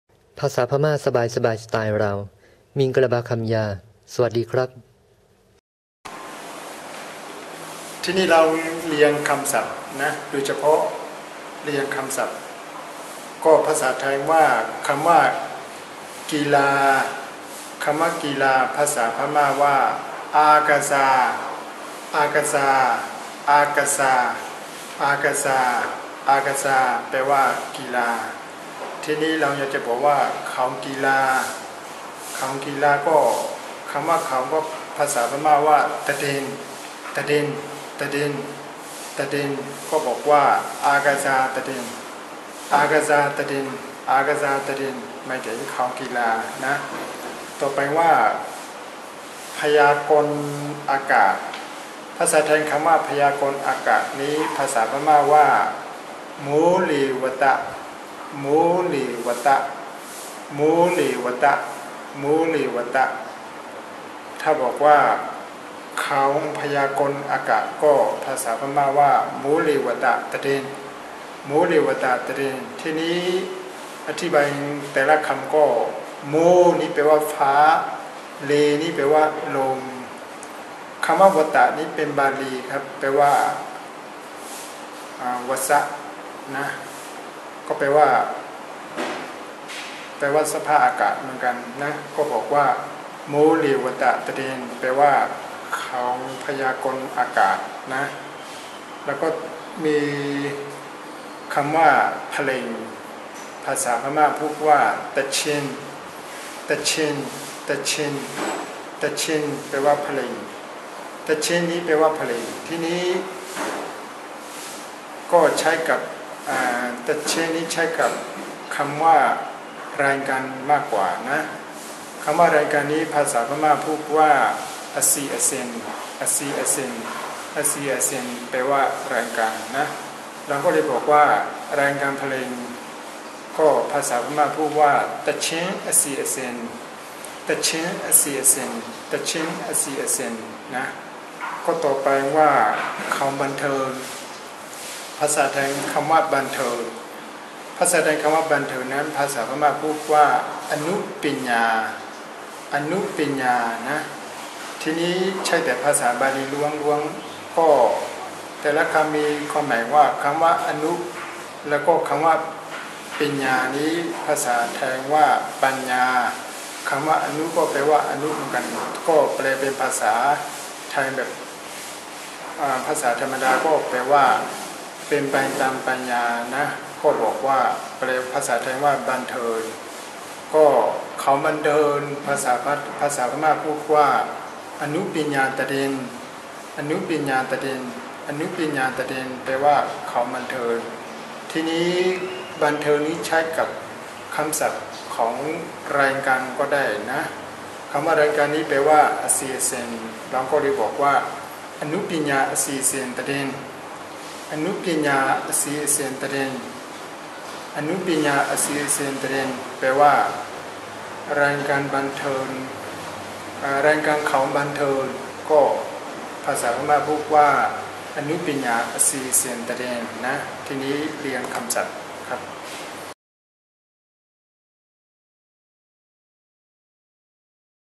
ภาษาพม่า ตอน112
ภาษาพม่า (เมียนมา) ตอน 112 - รายการวิทยุ // 'Learn Myanmar (Burmese / Burma language)' for Thai speakers. CC: BY-NC-SA.